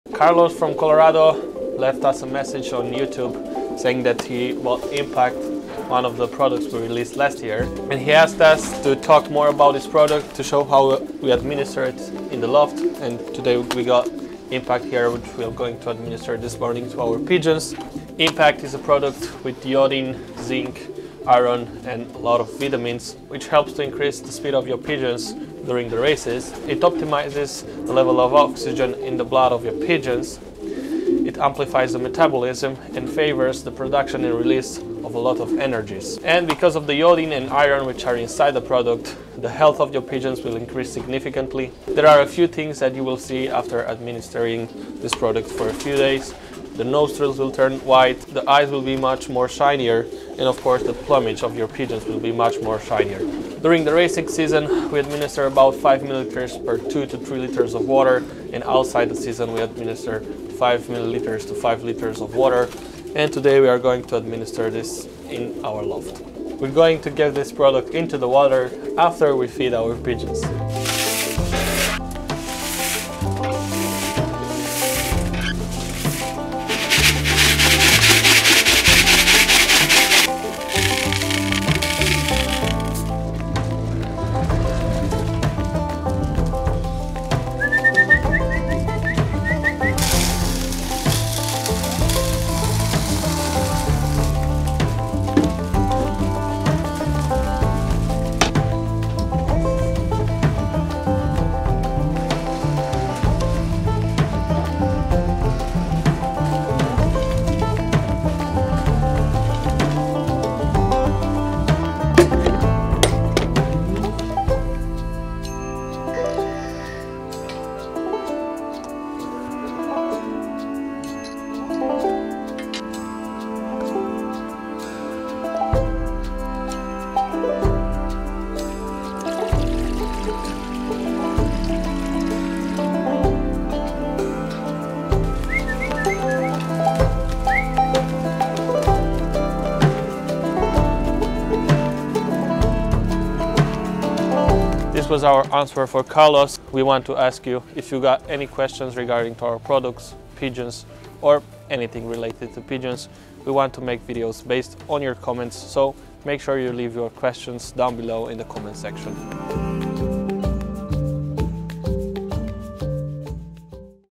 PIGEON INSIGHTS Q&A EP1 - “Do a video about Impact“
Today we're starting a series where we respond to your questions regarding raising pigeons. It's going to be called PIGEON INSIGHTS Q&A and we're starting the series with a question from Carlos who lives in Denver, Colorado asking for more details about Impact. Leave your questions in the comment section so we can respond.